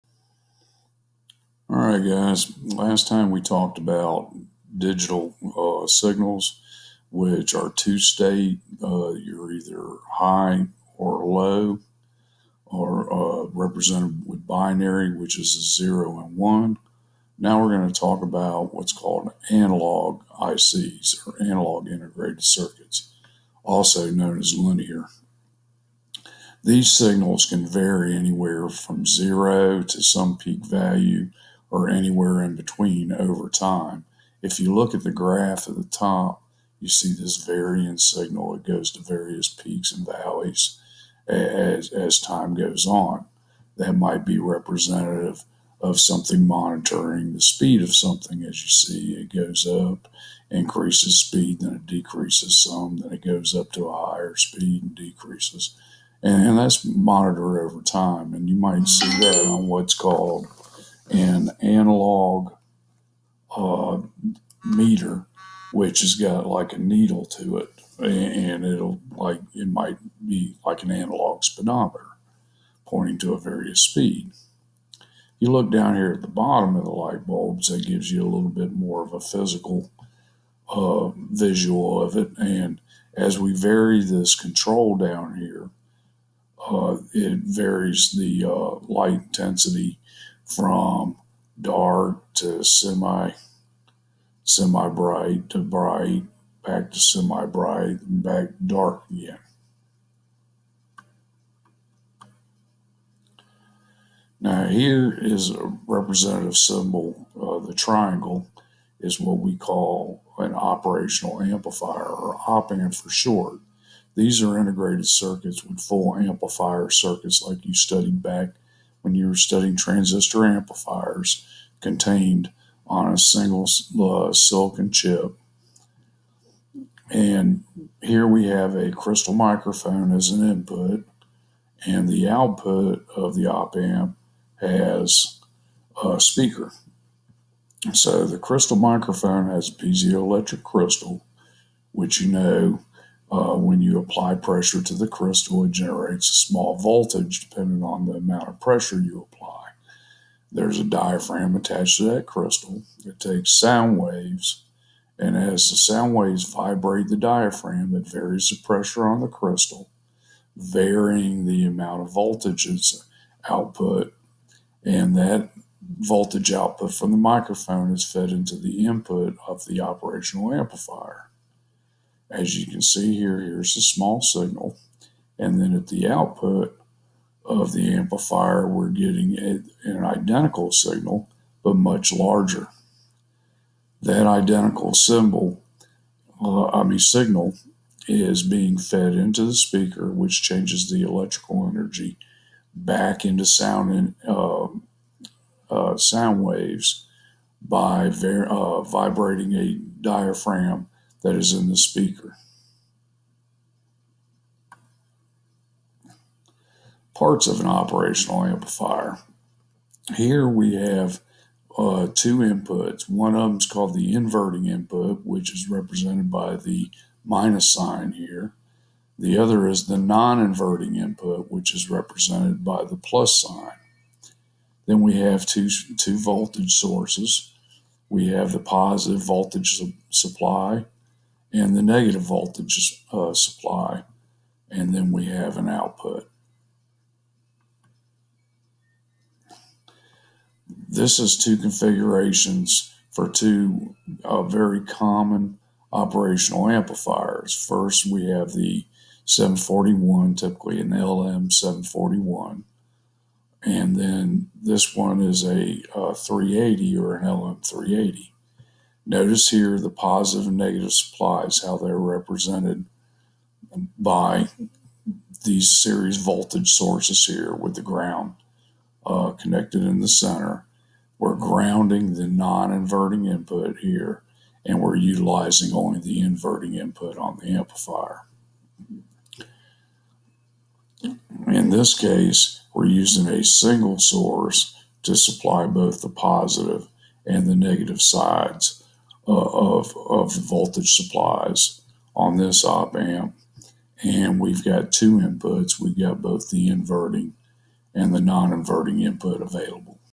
Integrated Circuits ICs Part III
Part III of a series of videos on integrated circuits. This video introduces analog ICs.